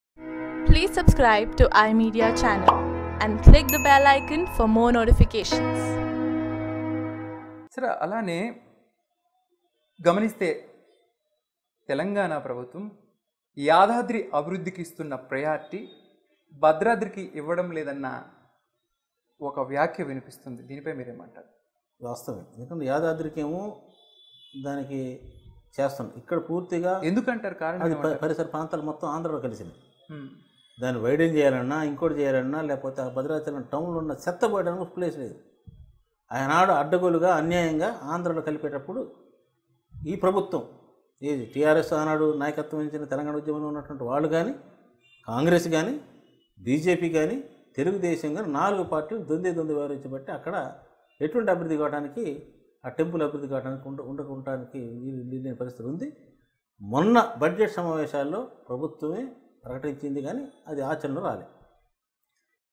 Why TRS Govt. Neglecting Bhadrachalam Development || MLA Sunnam Rajaiah || i Media
Badrachalam MLA Sunnam Rajaiah Face to Face with i Media

Sunnam Rajaiah is 58 years old MLA elected from Bhadrachalam (ST) of Andhra Pradesh state as a member of Communist Party of India (Marxist). Sunnam Rajaiah was born to Sunnam Rajulu.

Sunnam Rajaiah is an Indian politician, from Telangana. He was elected as a Member of Legislative Assembly of Andhra Pradesh in 2004,2014 and 2009 as a CPI(M) candidate.[2] In 2009 election he lost the seat to his opponent Kunja Satyavati by 6956 Votes.[